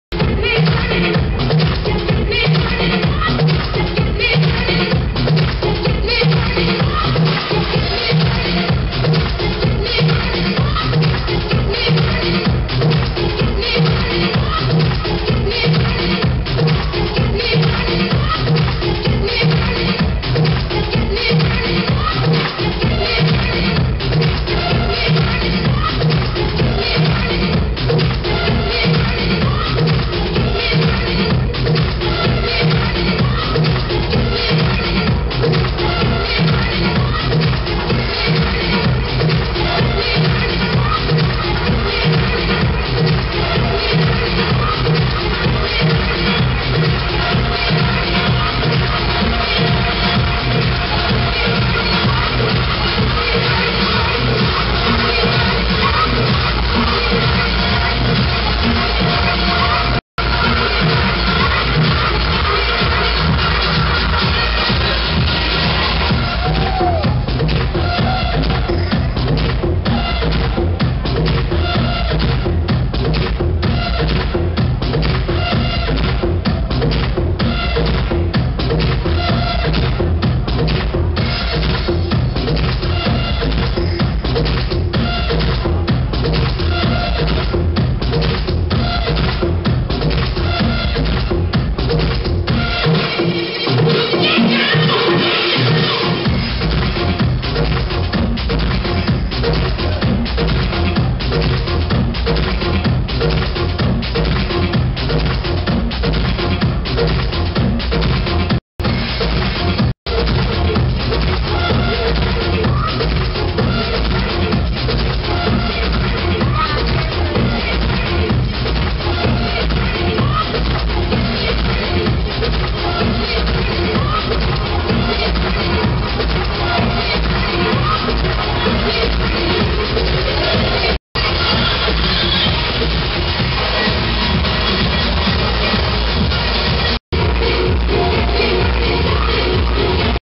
Darius Soli Performs with Fire Poi at Club One(Excuses) New Years Eve 2009
Darius Soli Performs with Fire Poi at Club One(Excuses) New Years Eve 2009 for Circuit Event in Deep Ellum(Dallas, TX).